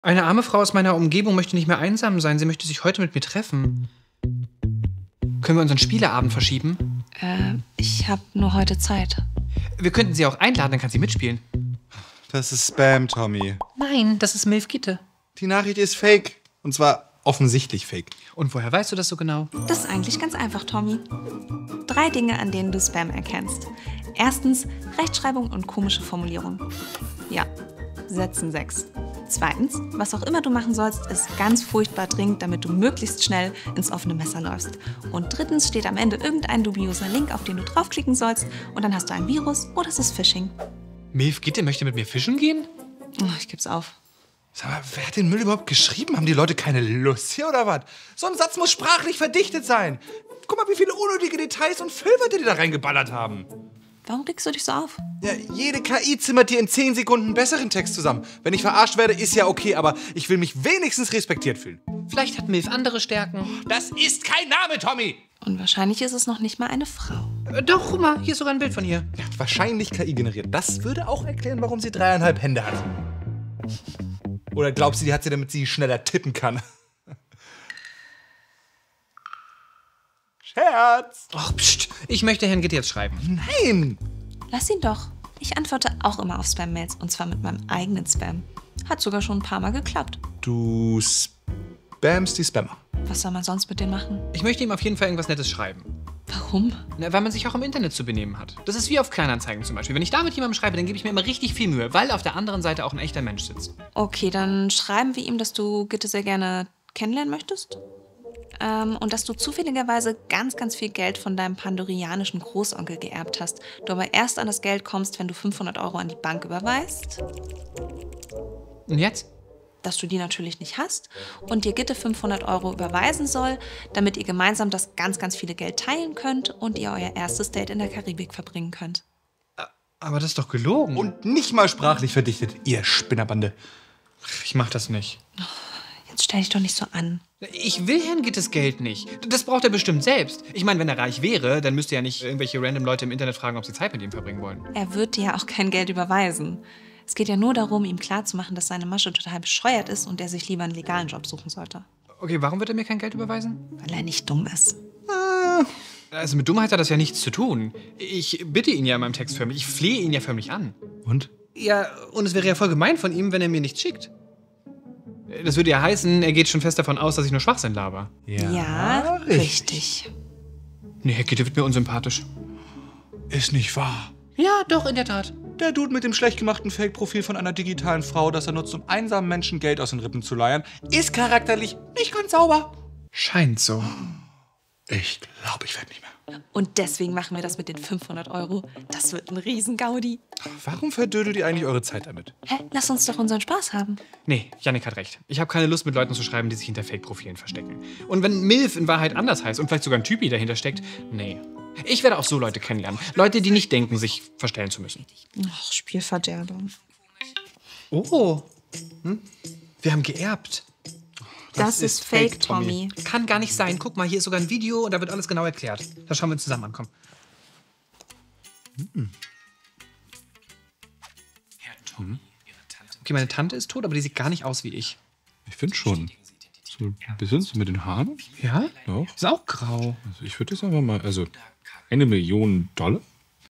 Wer fällt bitte heute noch auf Spam rein?
Spam-Mails waren vielleicht eine clevere Falle, als meine Omma noch jung war, aber heutzutage fällt da doch niemand mehr drauf rein … dachte ich! Aber kennt ihr meinen Mitbewohner Tommy? Naja, seht selbst! euer Jannik

Was hat das mit Kleinanzeigen zu tun?